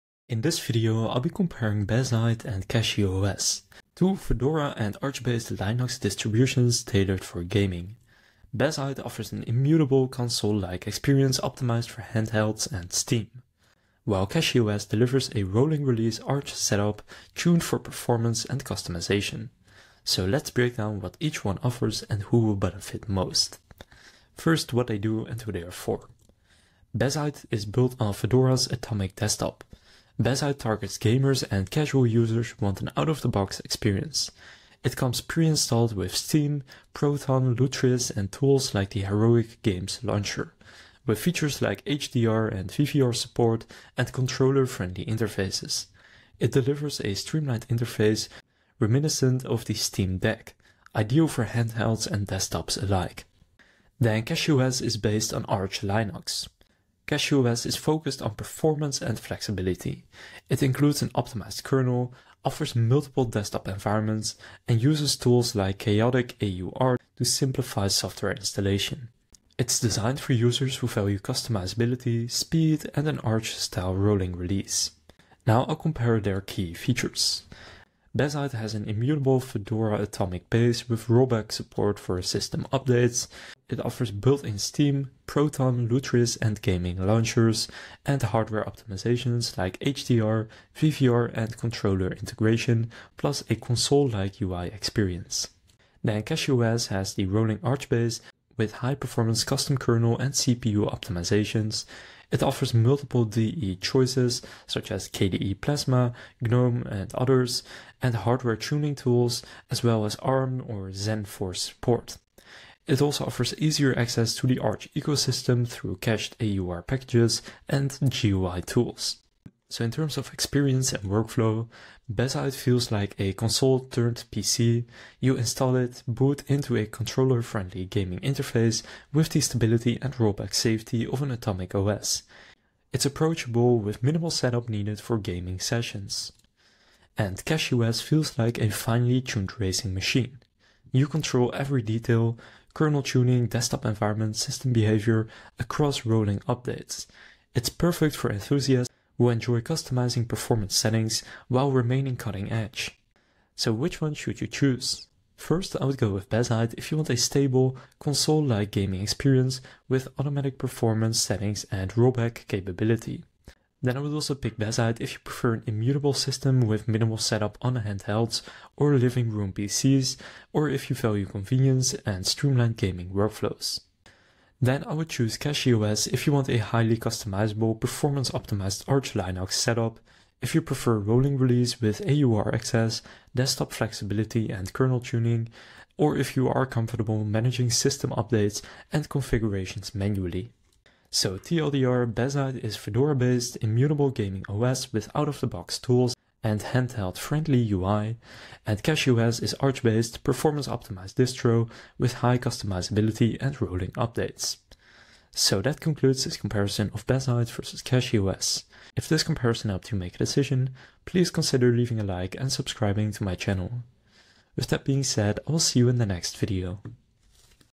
Bazzite Vs CachyOS (2026) | Which Gaming Linux Distro Is Better?
In this video, I’ll compare Bazzite Vs CachyOS.

So if you're wondering whether Bazzite or CachyOS is the better choice for gaming, this comparison is for you!

👉 We’ll compare their gaming performance, hardware compatibility, desktop environments, and which Linux distro provides the smoothest experience for gamers.